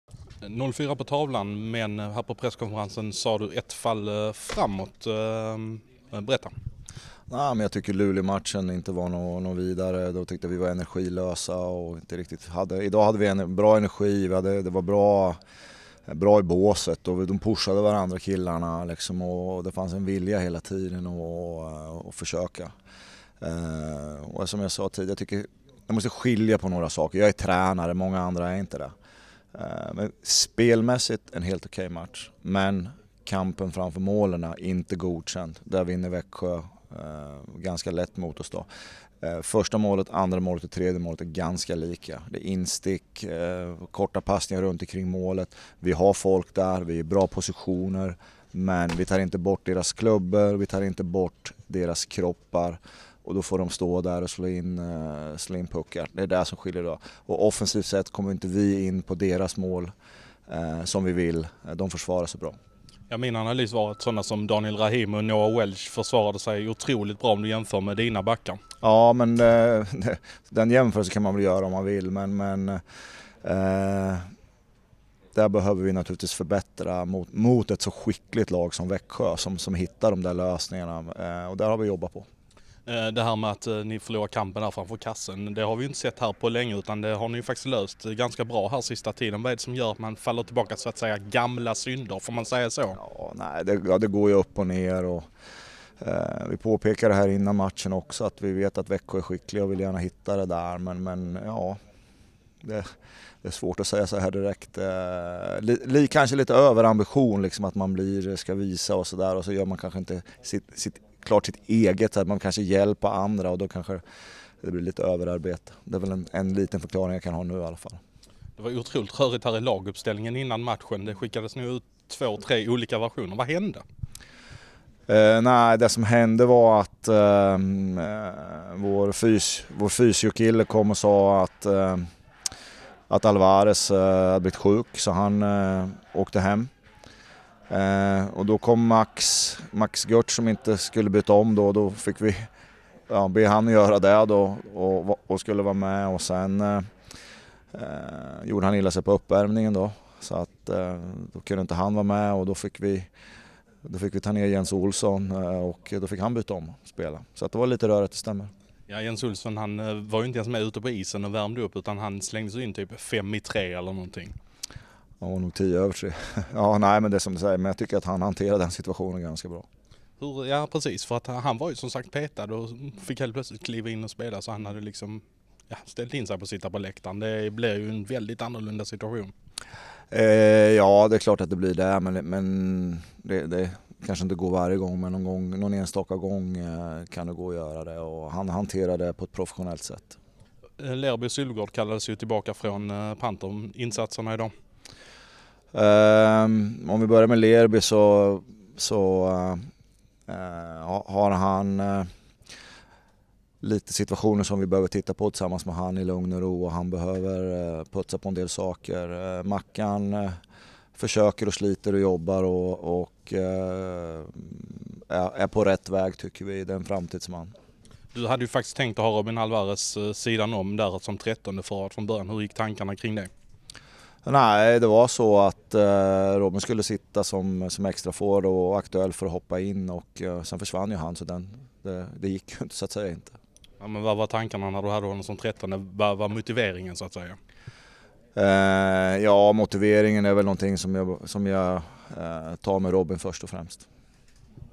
MrMadhawk.se Peter Andersson efter Malmö - Växjö 2018-01-13
Malmös tränare Peter Andersson efter matchen mot Växjö den 13 januari 2018. Resultat 0-4.